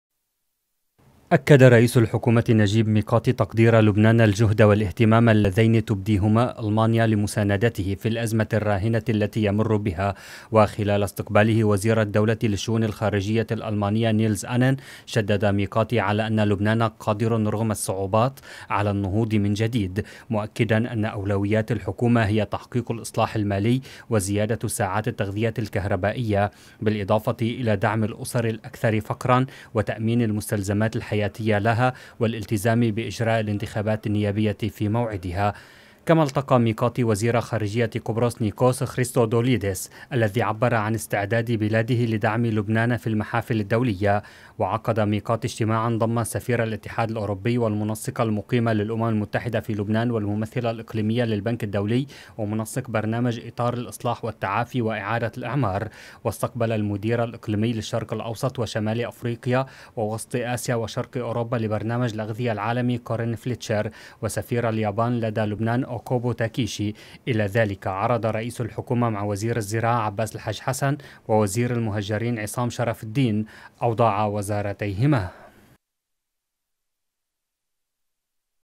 Lübnan'ın yeni Başbakanı Mikati, büyükelçilerle bir araya geldi
Lübnan Başbakanı Necib Mikati, bazı ülkelerin büyükelçileri ve diplomatlarıyla bir araya geldi.
Mikati, söz konusu yabancı misyon şefleriyle Lübnan'a destek yollarını görüştü.

لبنان -  ميقاتي يبحث مع عدد من السفراء والدبلوماسيين سبل دعم لبنان